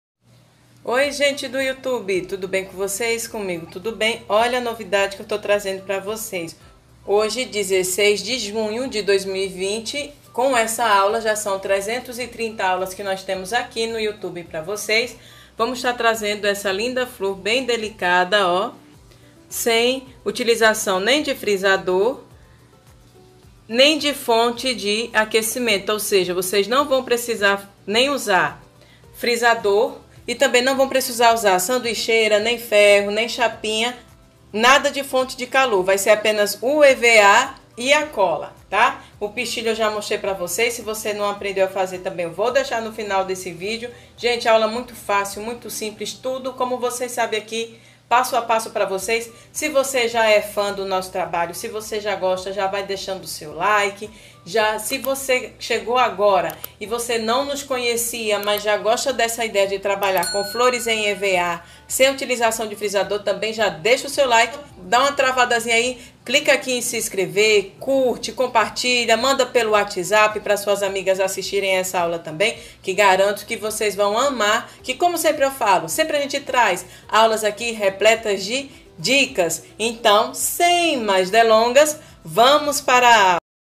FLOR CRISTAL SEM FRISADOR
Aprenda fazer uma linda cristal sem frisador